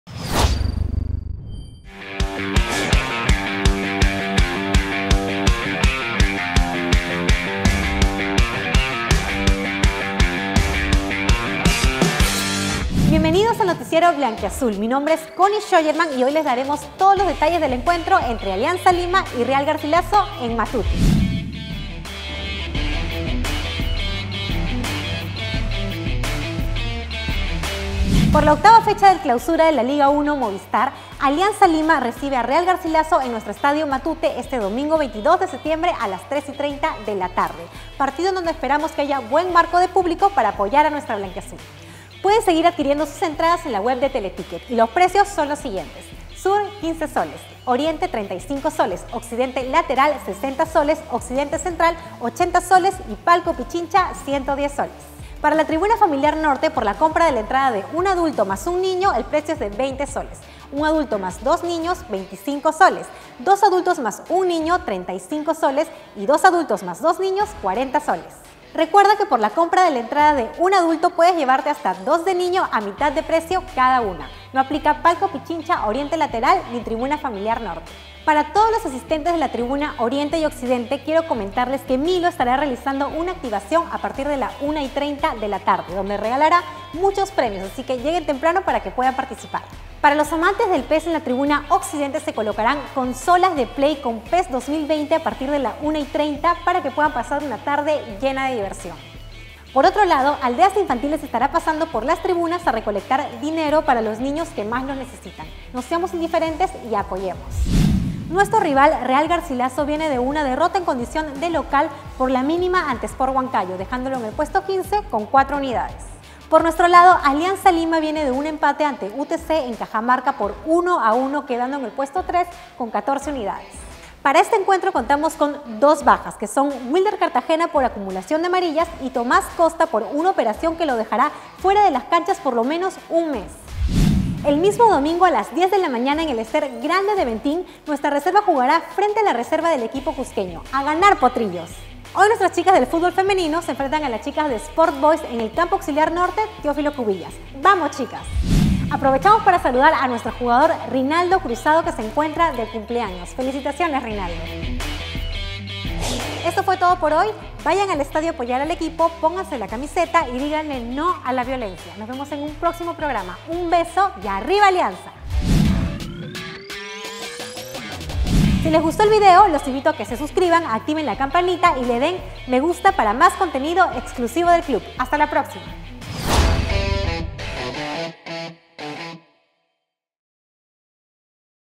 Programa informativo 20 | NOTICIERO BLANQUIAZUL
En nuestro programa 20 te presentamos todos los detalles previos al encuentro ante Real Garcilaso por el Torneo Clausura en Matute. Conocerás las novedades en el once titular y una gran sorpresa para este domingo. Divisiones Menores, Fútbol Femenino y más. ¡No te lo pierdas!